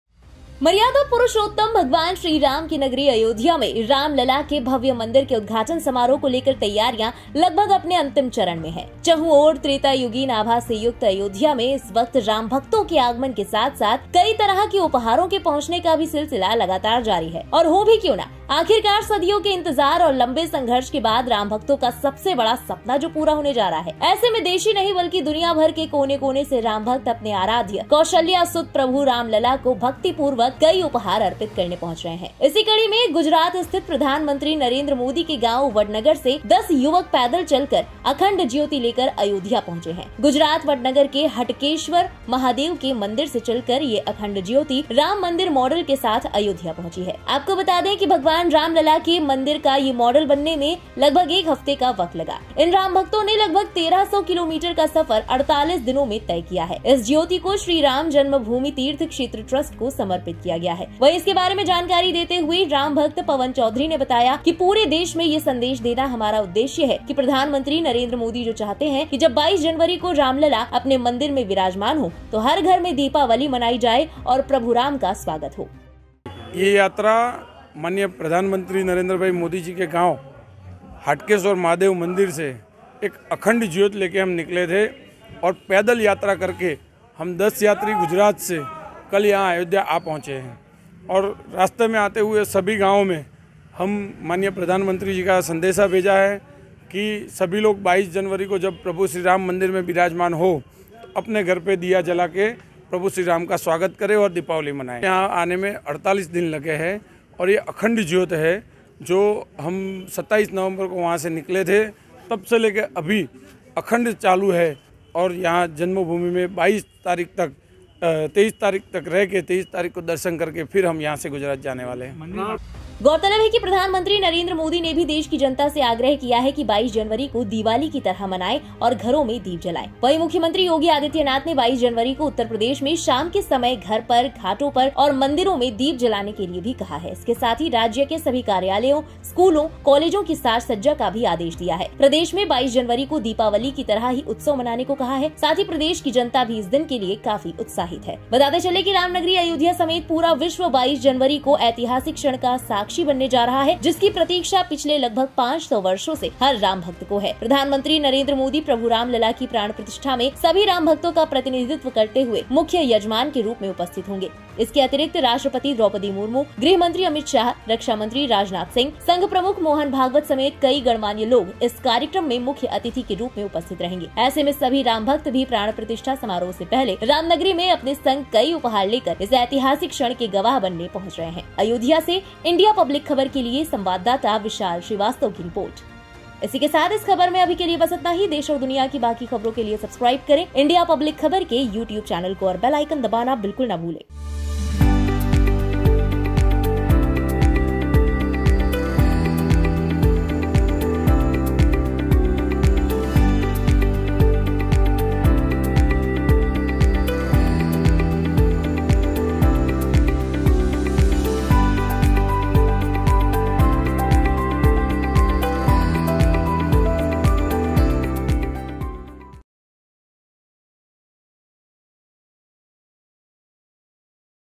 10 युवकों ने 1300 KM पैदल तय किया Ayodhya का सफर, PM Modi के गाँव से पहुंचाई अखंड ज्योति
10 युवकों ने 1300 KM पैदल तय किया Ayodhya का सफर, PM Modi के गाँव से पहुंचाई अखंड ज्योति

गुजरात स्थित प्रधानमंत्री नरेंद्र मोदी के गांव वडनगर से 10 युवक पैदल चलकर अखंड ज्योति लेकर अयोध्या पहुंचे हैं. गुजरात वडनगर के हटकेश्वर महादेव के मंदिर से चलकर यह अखंड ज्योति राम मंदिर मॉडल के साथ अयोध्या पहुंची है. भगवान राम लाल का मंदिर मॉडल मे एक हफ्ता बनाने में लगा था..इन रामभक्तों ने लगभग 1300 किलोमीटर का सफर 48 दिन में तय किया है. इस ज्योति को श्रीराम जन्मभूमि तीर्थ क्षेत्र ट्रस्ट को समर्पित किया गया है।

10 youths from Gujarat-based Prime Minister Narendra Modi's village Vadnagar have reached Ayodhya on foot carrying Akhand Jyoti. Starting from the temple of Hatkeshwar Mahadev in Vadnagar, Gujarat, this Akhand Jyoti has reached Ayodhya with the Ram temple model. It took a week to build the model of Lord Ram Lal's temple. These Ram devotees have covered a distance of about 1300 kilometers in 48 days. This flame has been dedicated to Shri Ram Janmabhoomi Teerth Kshetra Trust.